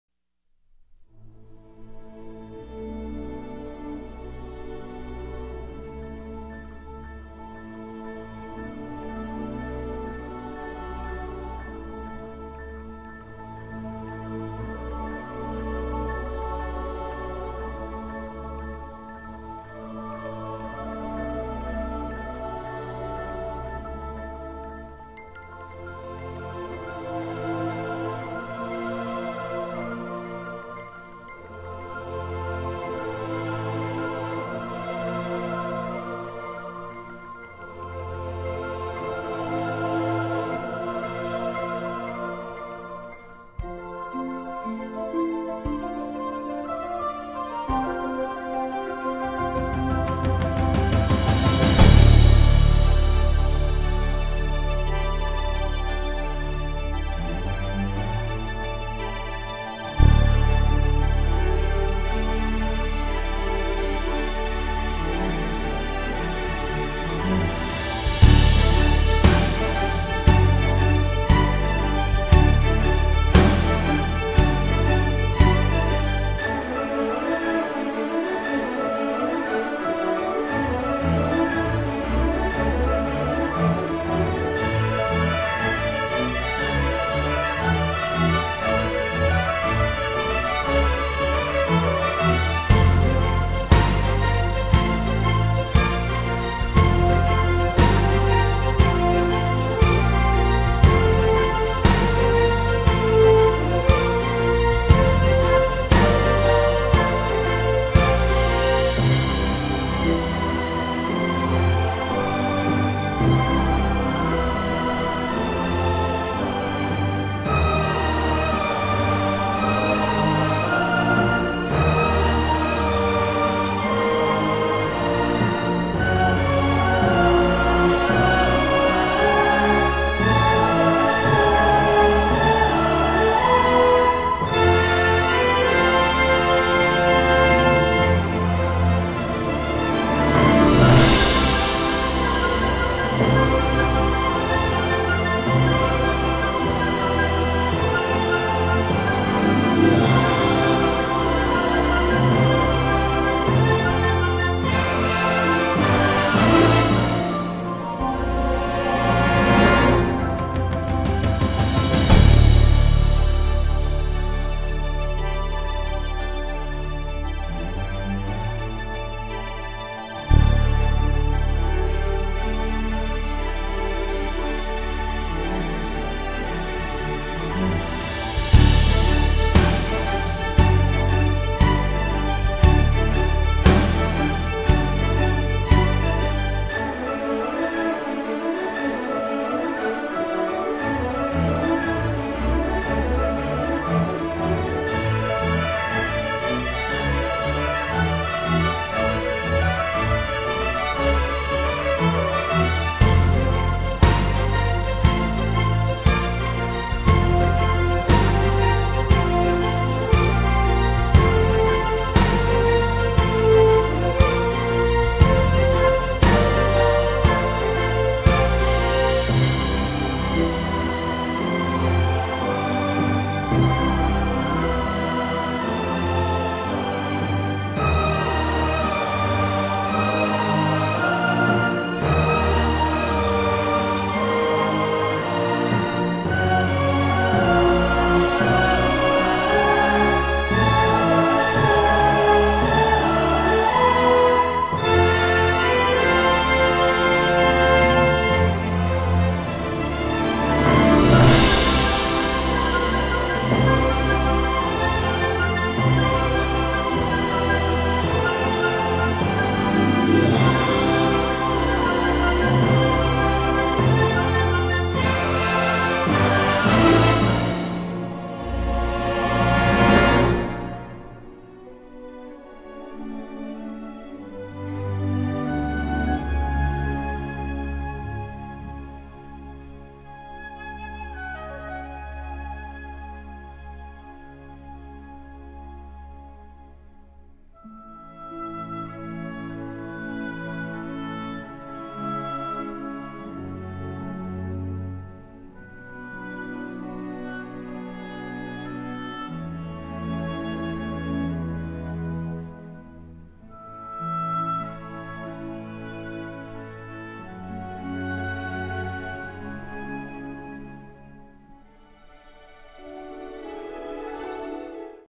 Castle Tour
Here is a short tour of my most recent wall mural project that I did over spring break. The painting was done for a little girl's bedroom. She dreams of being a princess.